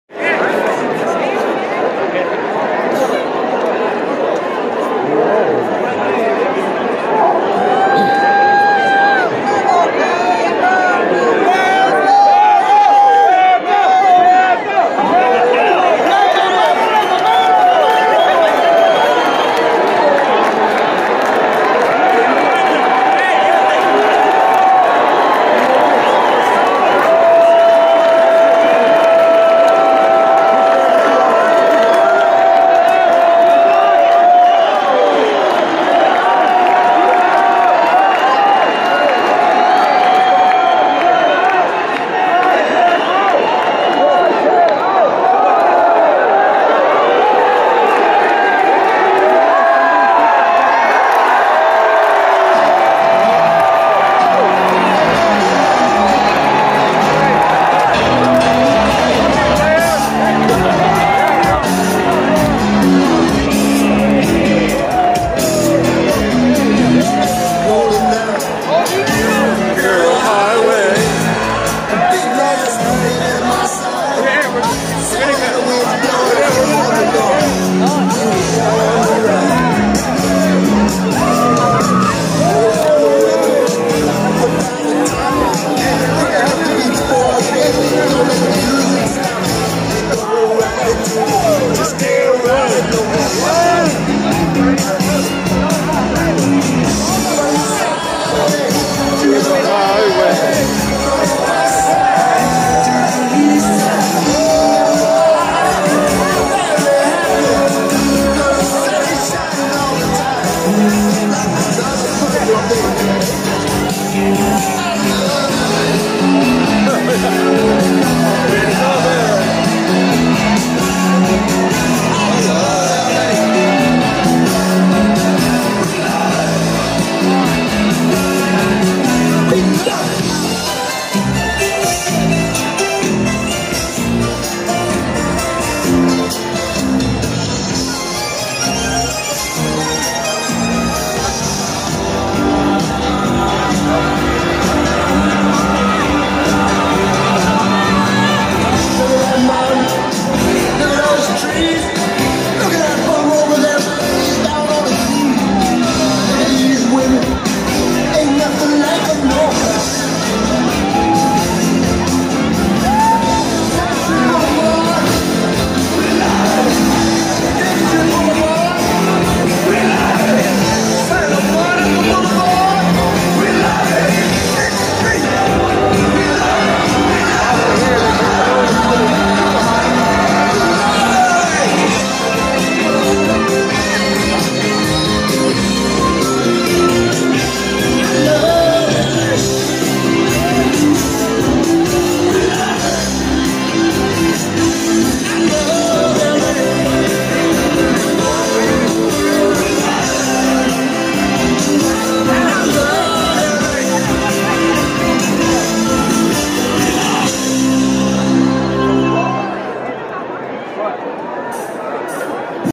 Dodgers beat the Diamondbacks 5 to 1
Dodgers Vs Diamondback